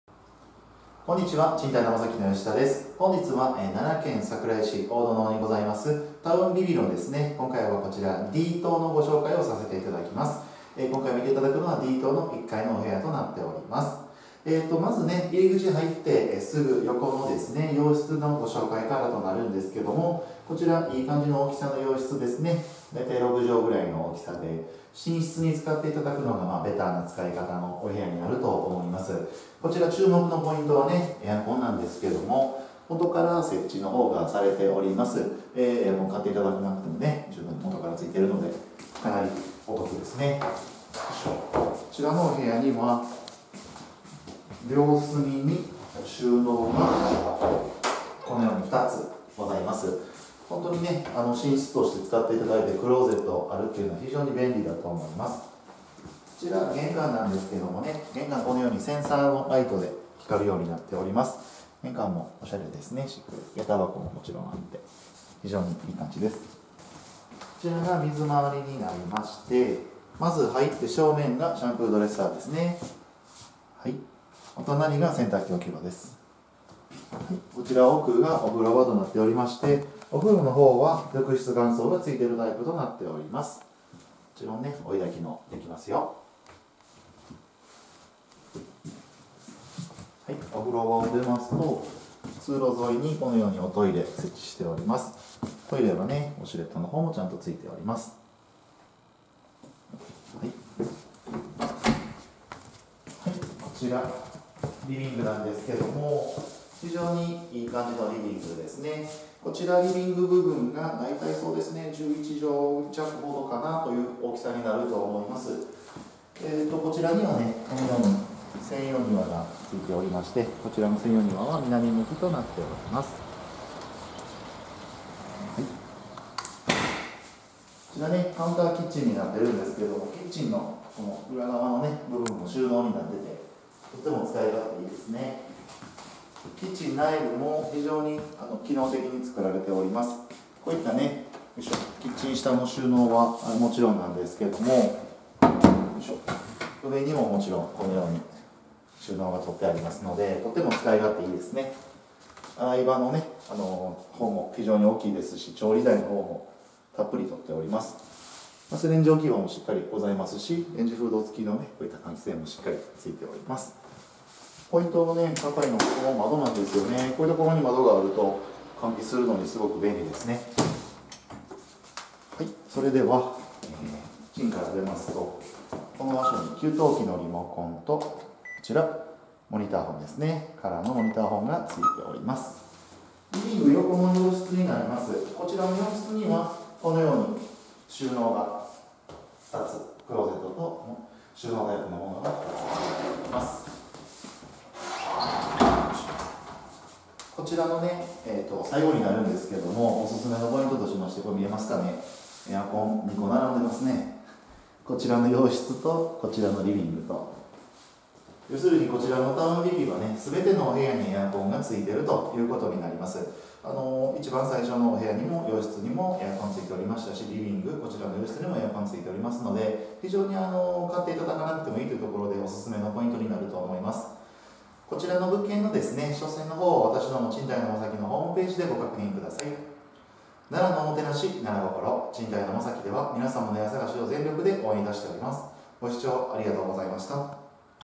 5709 タウン ビビＤ ♪ 賃貸のマサキ
桜井市粟殿町にあります2LDKアパートの【タウン　ビビＤ】の紹介動画になります。